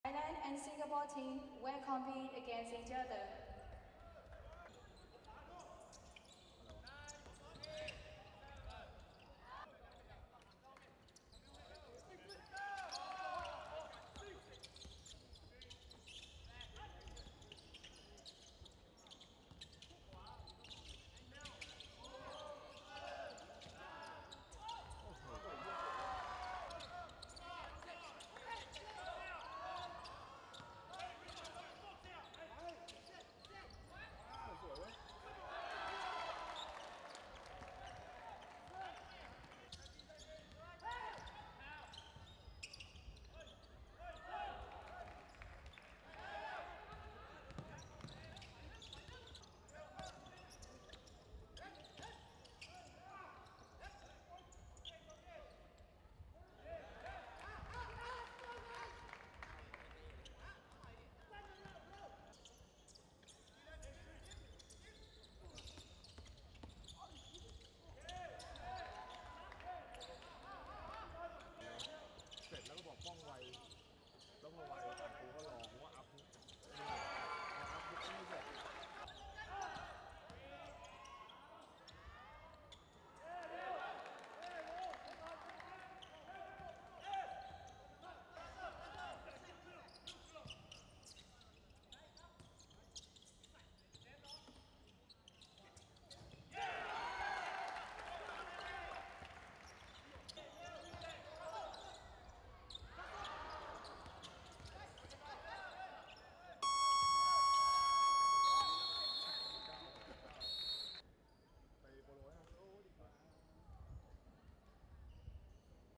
ไฮไลท์บาสเกตบอลชาย ไทย ชนะ สิงคโปร์ 69-59
ไฮไลท์การแข่งขันกีฬาซีเกมส์ ครั้งที่ 27 ที่ประเทศเมียนมาร์ : บาสเกตบอลชาย ไทย ชนะ สิงคโปร์ 69-59